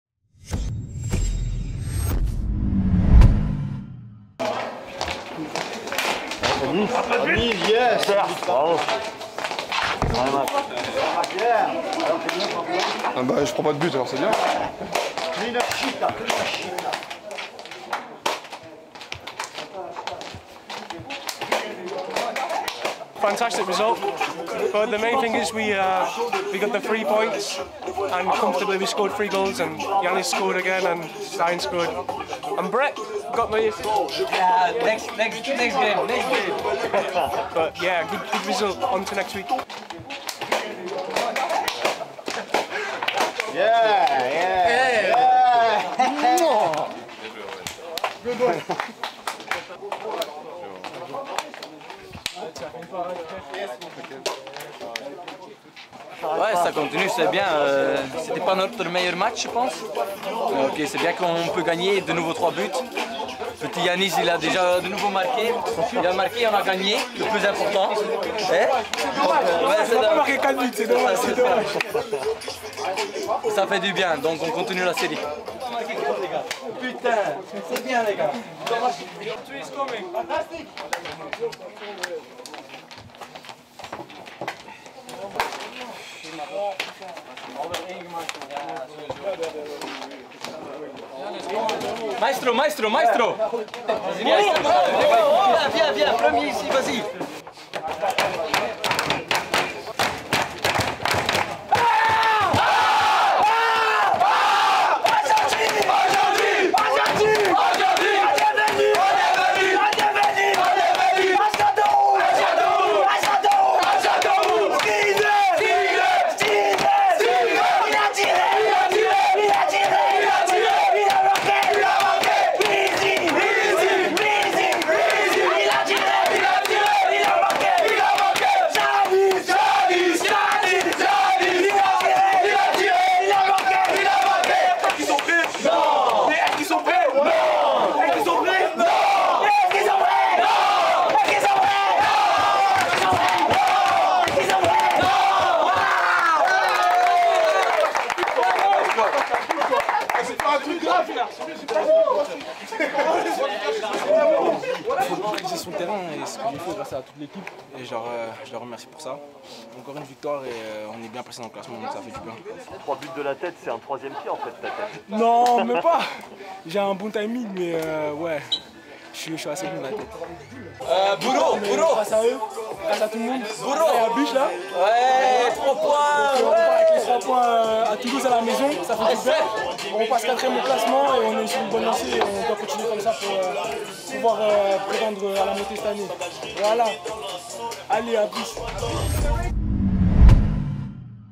LBCTFC Dans l'intimité du vestiaire toulousain après le succès à Châteauroux
Revivez la joie du vestiaire toulousain après la victoire face à La Berrichonne de Châteauroux (3-0) lors de la 15ème journée de @ligue2fr et découvrez les réactions des buteurs Rhys Healey et Janis Antiste, ou encore du milieu de terrain Brecht Dejaegere.